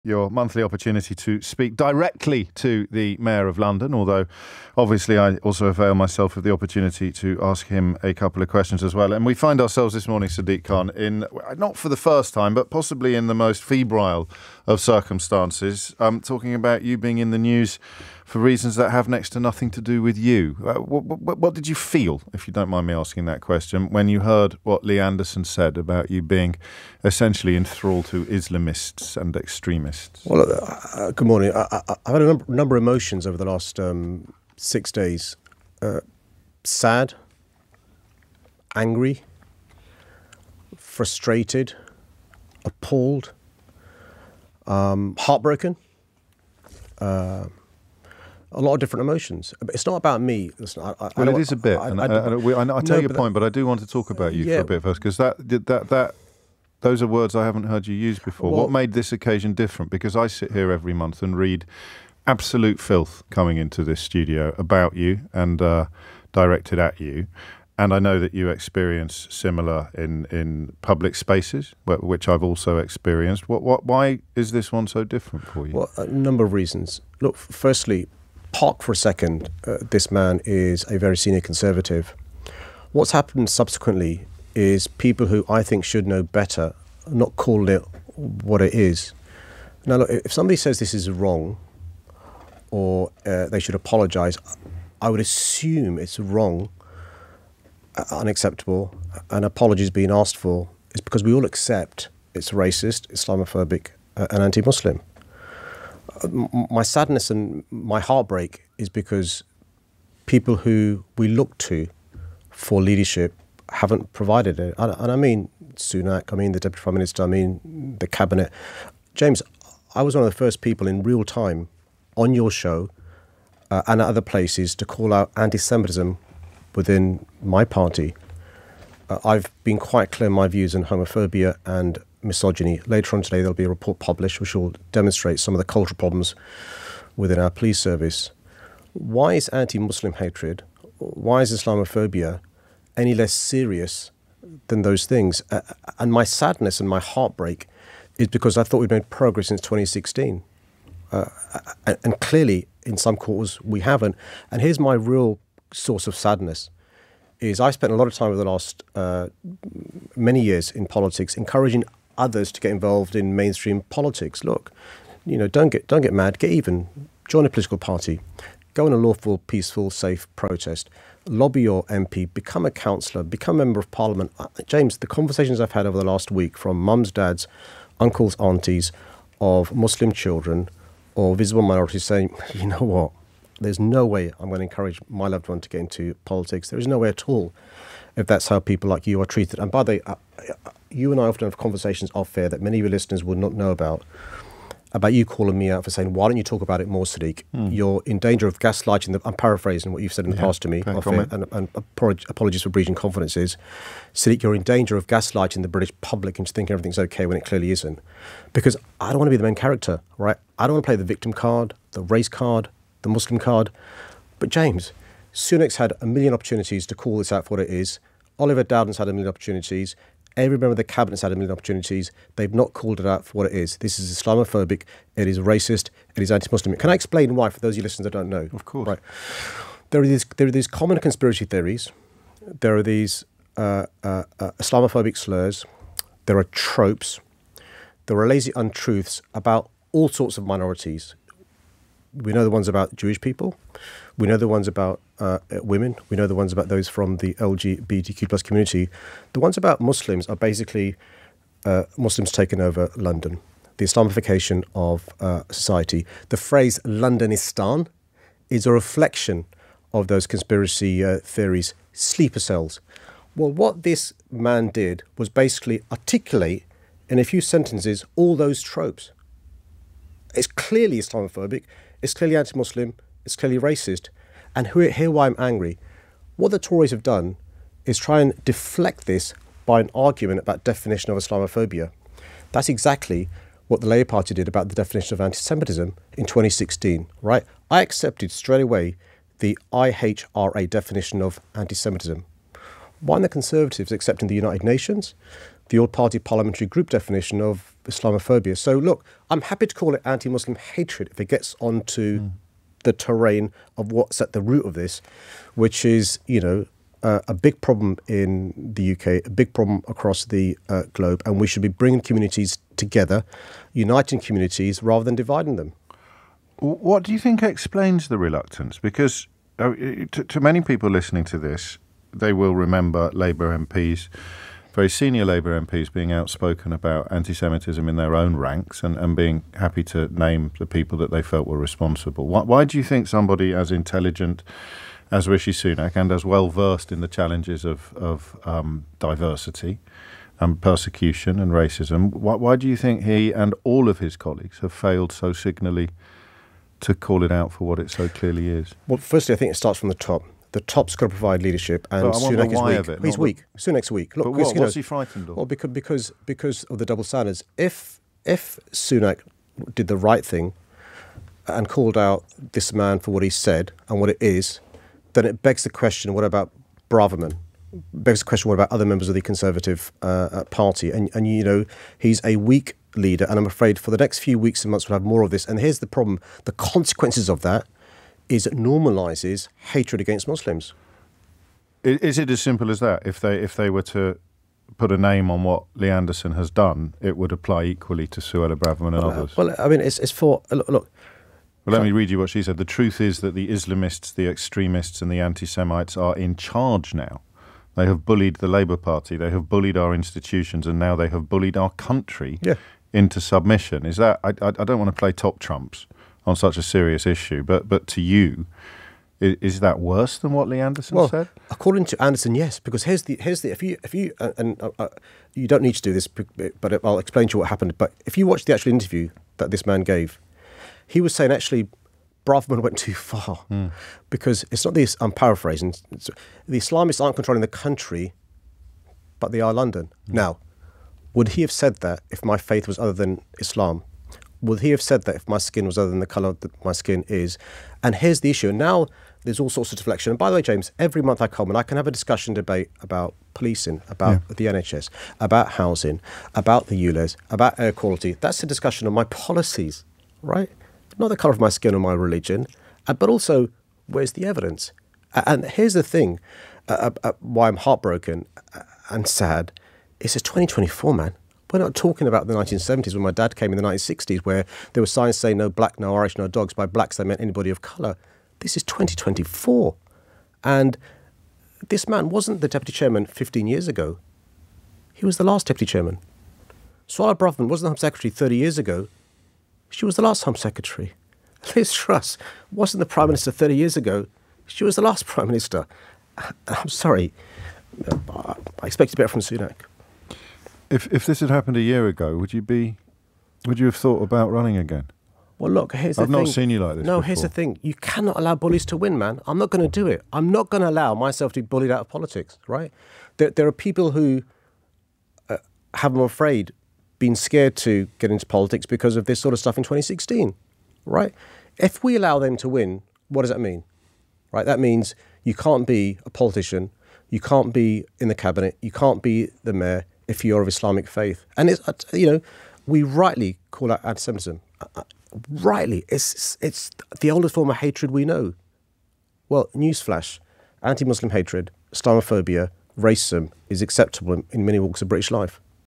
Lee Anderson response shows 'Rishi Sunak is weak,' says Sadiq Khan | LBC
Sadiq Khan tells James O'Brien that the consequences of ‘weak Rishi Sunak’ refusing to label ‘Islamist’ comments Islamophobic will be a rise in anti-Muslim hatred.

The London Mayor was referencing recent comments directed his way from Lee Anderson, who claimed "Islamists" had "got control" of him. Anderson has subsequently been suspended from the Conservative Party - but Khan stresses that the Prime Minister should still have gone further in criticising the Tory MP.

Khan also tells James that former Home Secretary Suella Braverman's rhetoric 'went further' than Anderson's and begs the question as to why she wasn't called out in the same way.